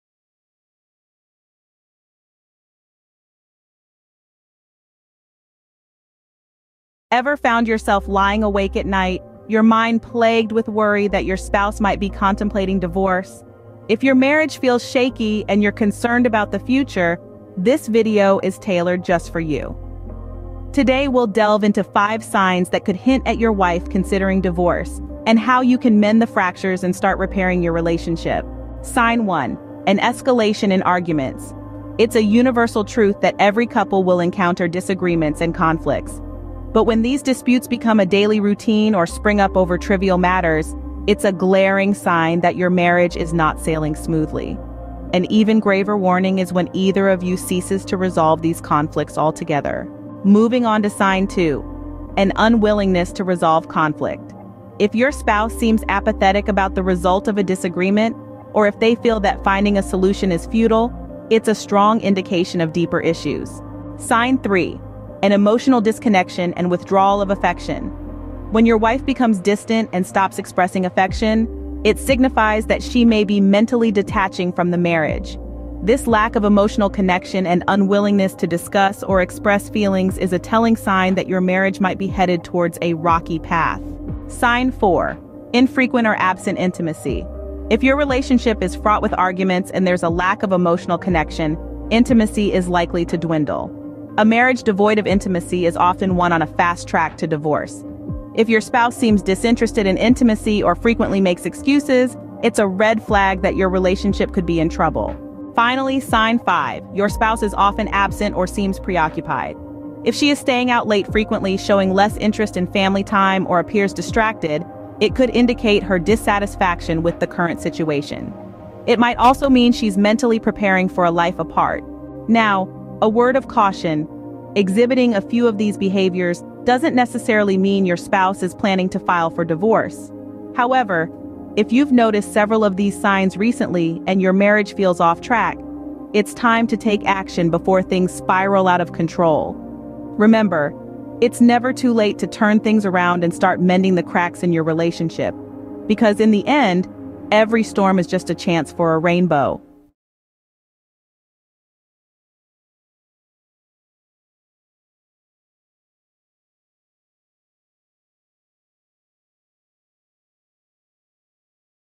5 Signs Your Wife Wants a Divorce
5 Red flags to show whether your wife is considering a divorce.